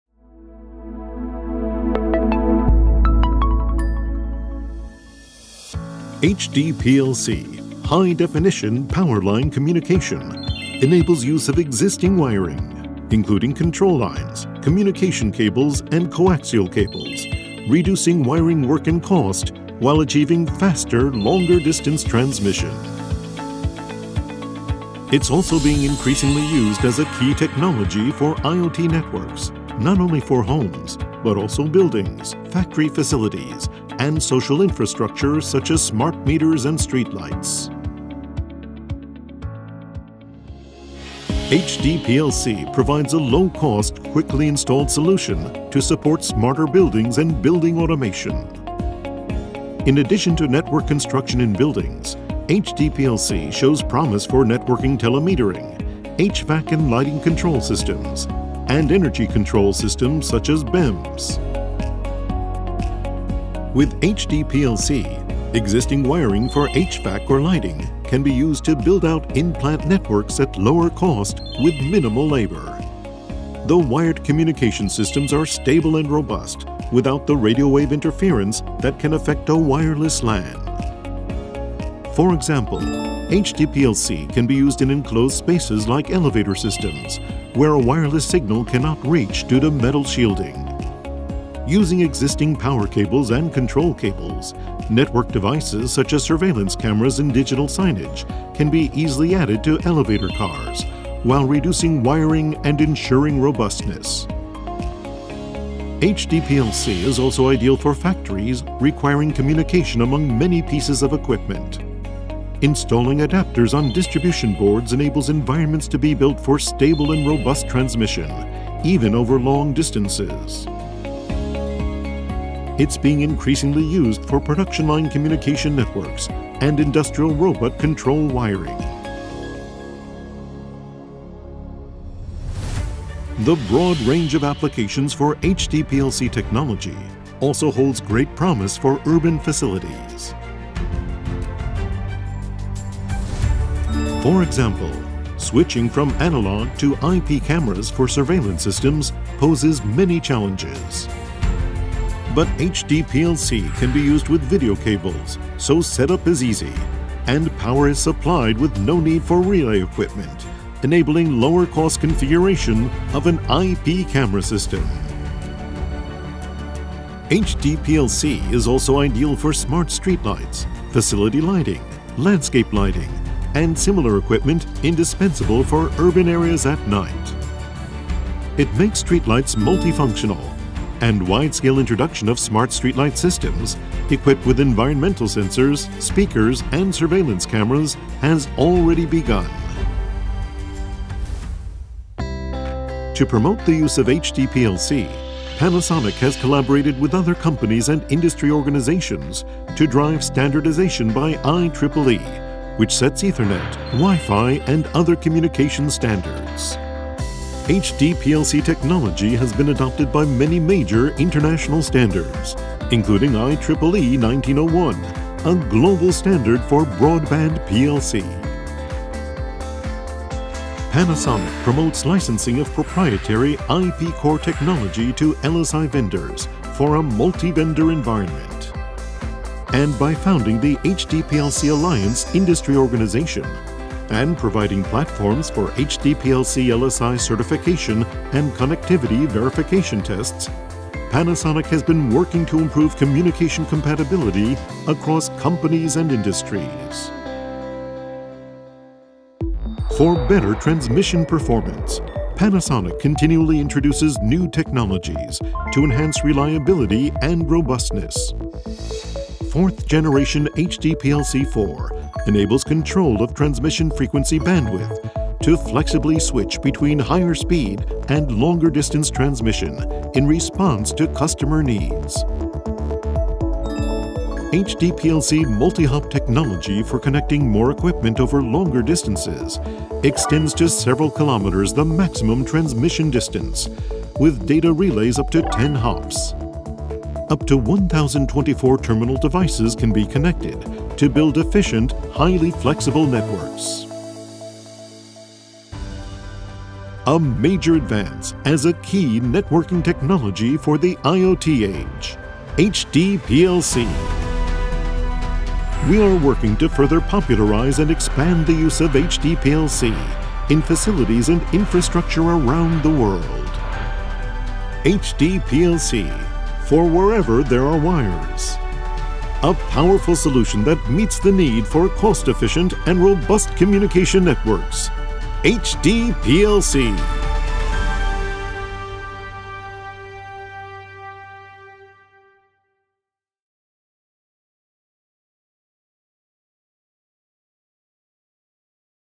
Power line communication "HD-PLC" Introduction (Nessum)
HD-PLC, one of the power line communication technologies, uses not only power lines but also any type of cables such as coaxial cable and twisted-pair cable to realize high-speed and long-range communication. The following is introduced in the video.
- Use Case
- 4th generation HD-PLC technology
- HD-PLC multi-hop technology
If you are considering speed-up the wired communication you are using now, please take a look. HD-PLC can make a hybrid network solution with wireless communication systems such as 5G, WiFi, and LPWA.

00:00　HD-PLC Overview
00:40　Use Cases
03:12　HD-PLC Technology